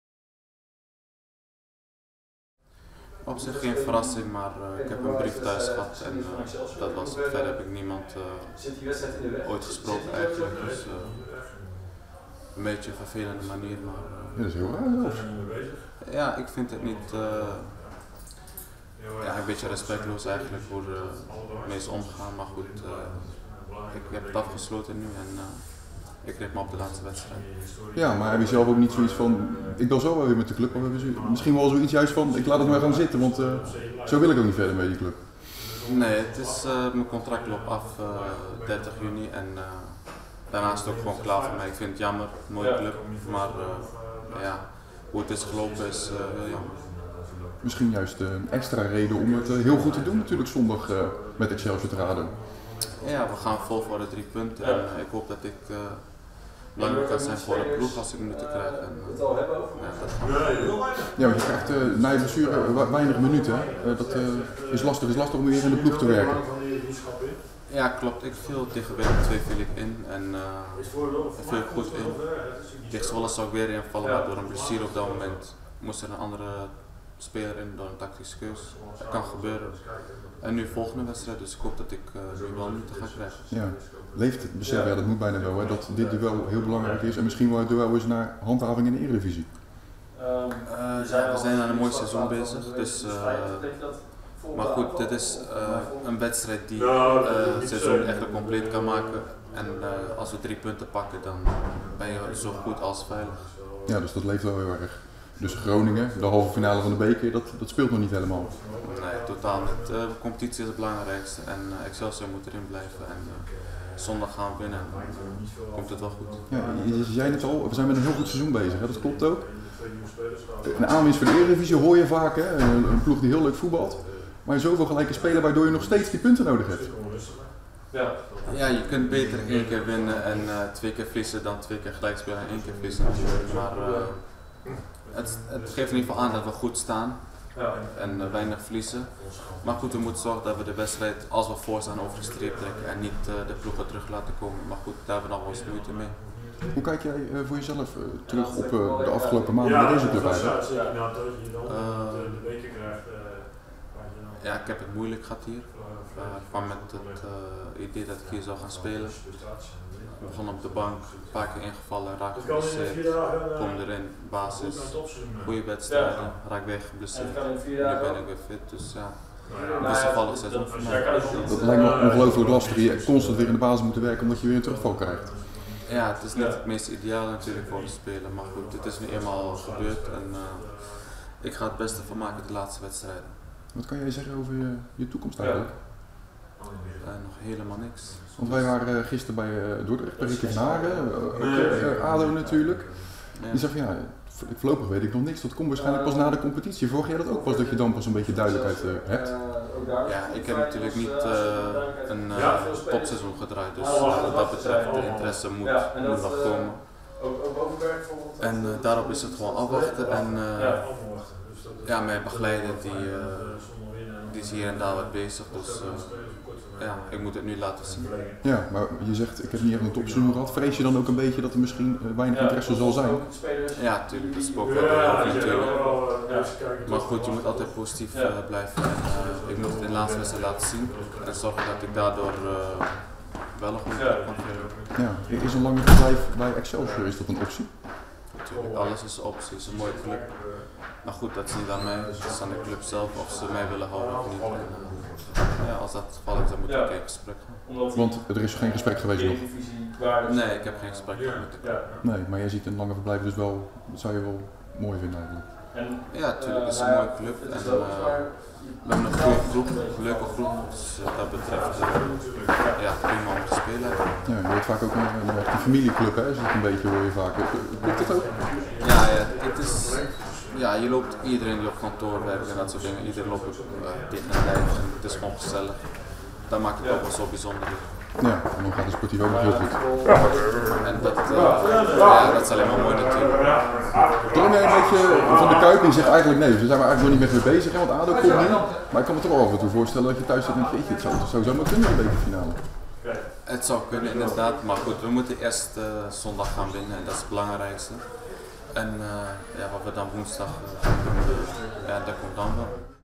Ninos Gouriye over vertrek bij ADO Den Haag
Excelsior kan zondag een hele belangrijke stap zetten richting lijfsbehoud. Als ADO Den Haag wordt verslagen op Woudestein spelen de Kralingers ook komend seizoen zo goed als zeker op het hoogste niveau. Dat beseft ook trainer Marinus Dijkhuizen zich. "Hoe sneller hoe beter," aldus de oefenmeester.

   Lees meer: Dijkhuizen: 'Dit is het duel om ons veilig te spelen'